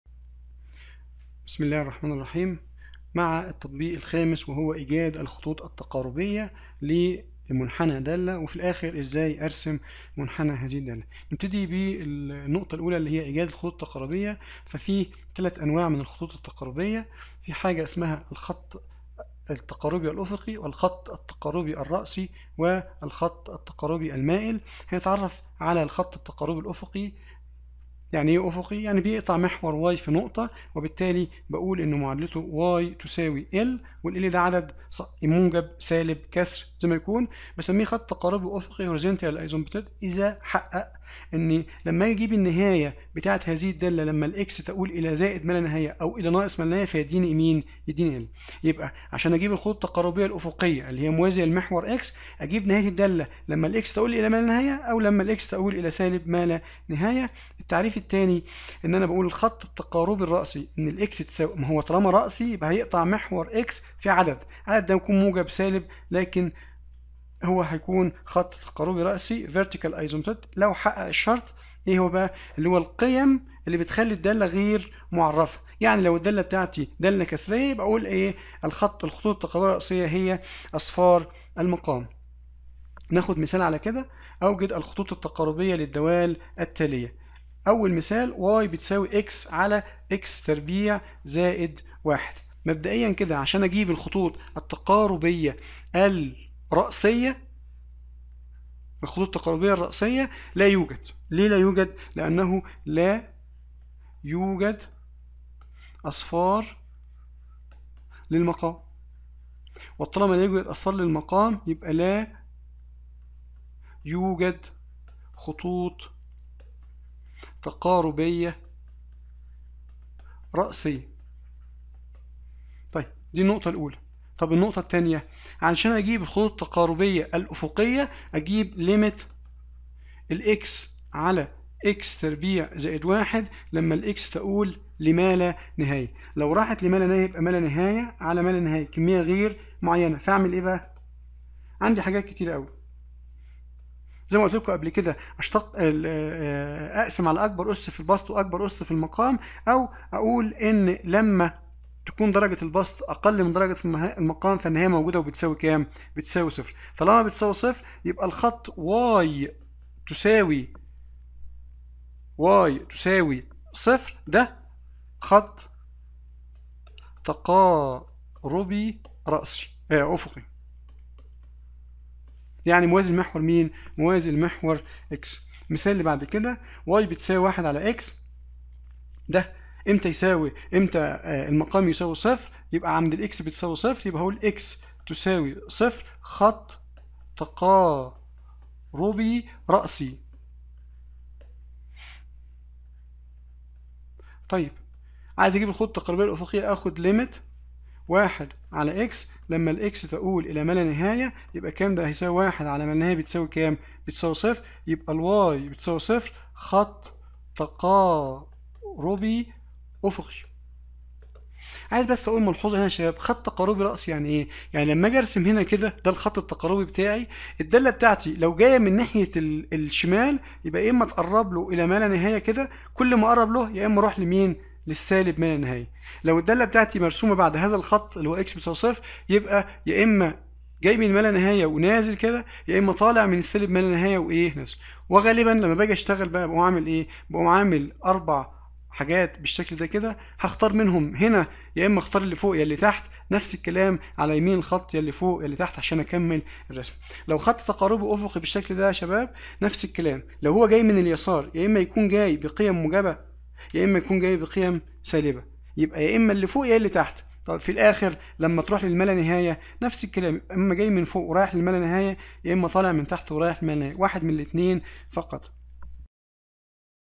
حساب التفاضل والتكامل - الوحدة 5 : الخطوط التقاربية ورسم المنحنيات - 1 - Asymptotes
المقررات المفتوحة
حساب التفاضل والتكامل(1) - 101 ريض
الوحدة الخامسة - دراسة تغير الدالة
الخطوط التقاربية الأفقية - الخطوط التقاربية الرأسية - أمثلة.